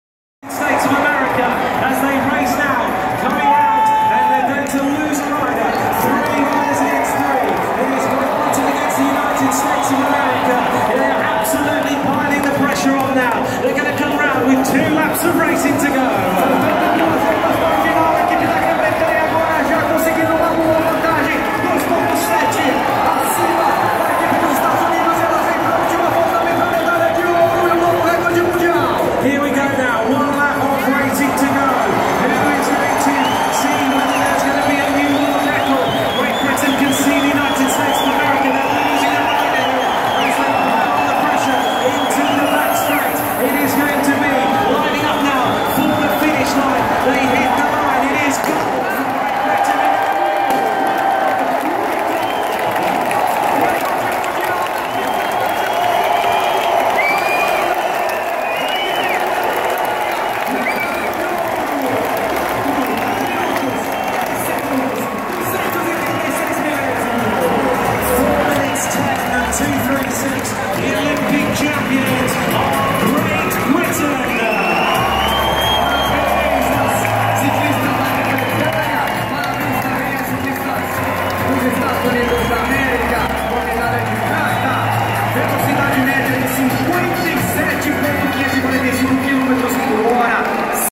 Olimpíadas Rio 2016 - Ciclismo Perseguição por equipes feminino
Ouro para a equipe britânica na prova de perseguição por equipes feminino na disputa contra as americanas que ficaram a prata no Velódromo.